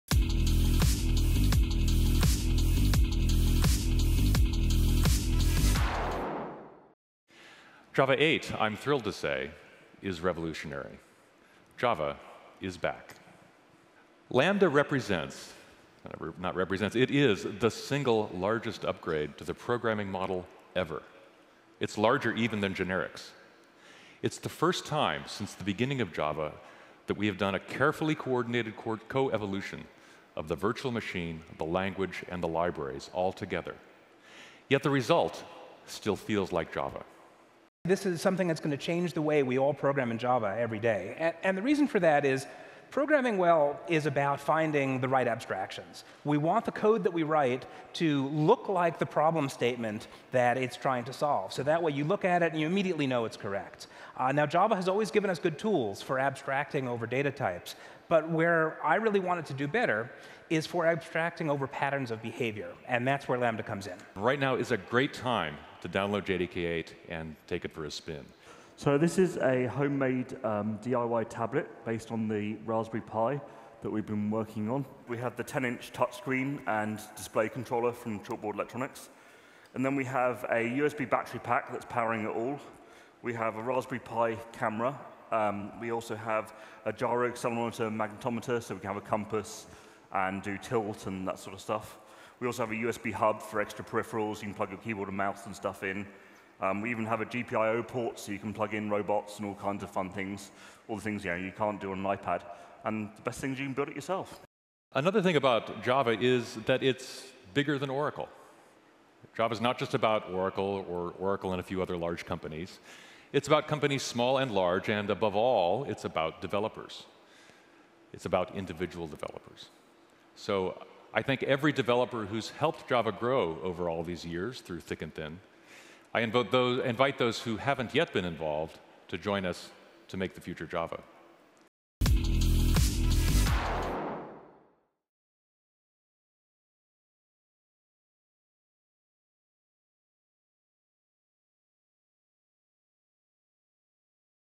JavaOne Technical Highlights 9-22-2013
View the highlights from the JavaOne Technical keynote at JavaOne 2013.